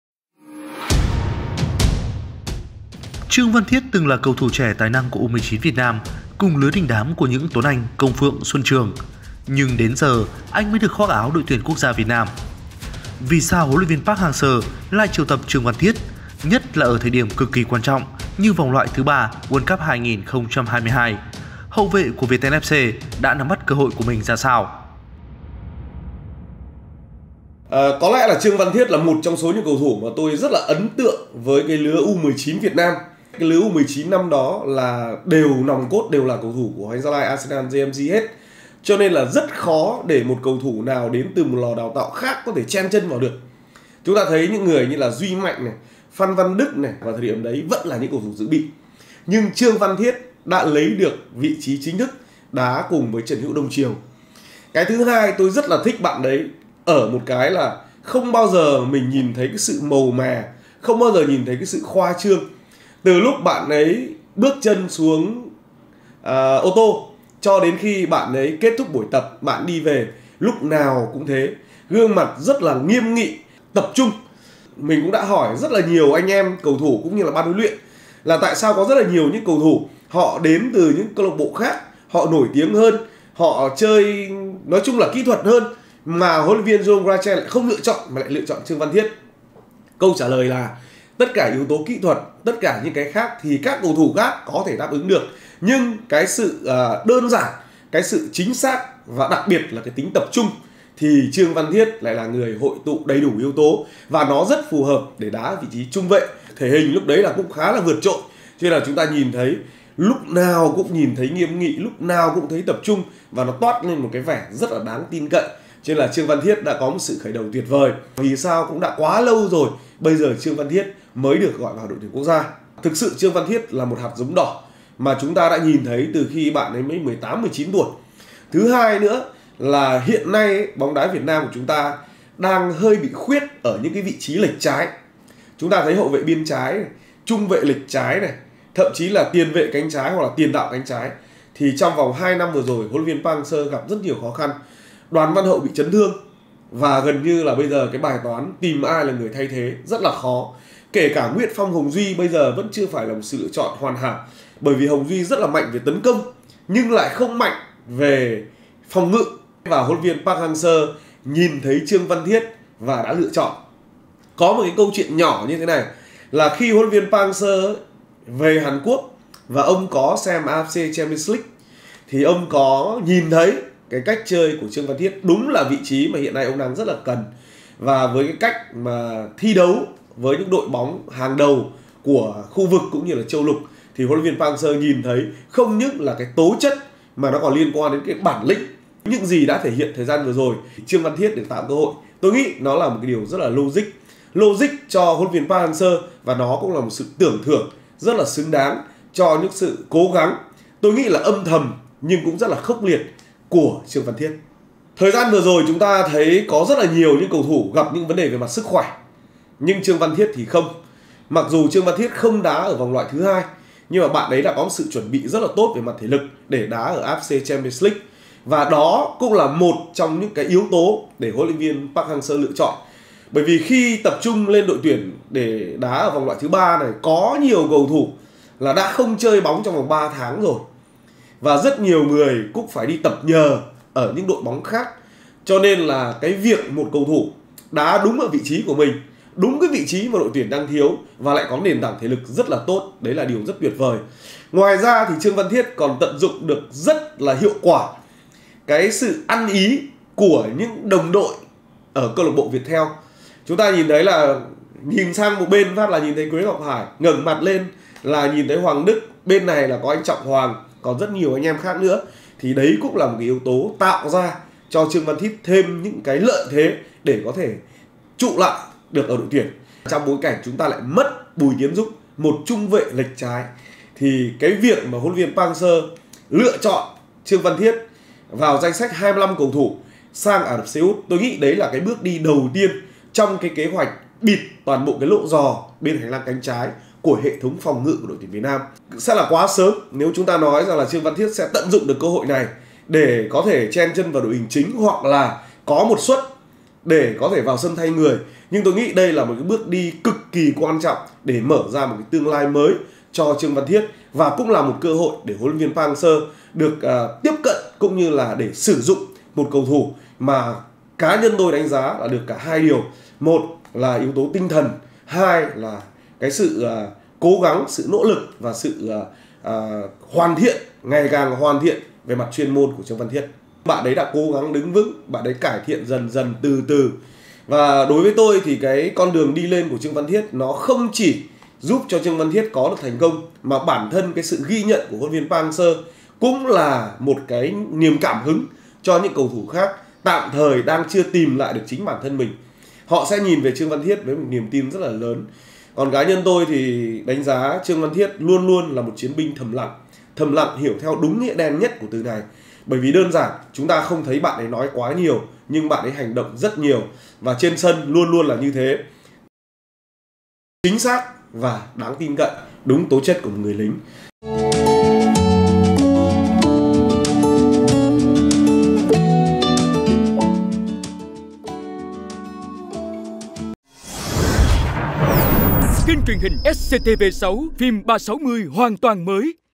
Tân binh của ĐTVN Trương Văn Thiết: Tìm cơ hội từ chiếc vé tới Ả rập Xê út
️Chuyện về hậu vệ Trương Văn Thiết- tân binh của ĐTVN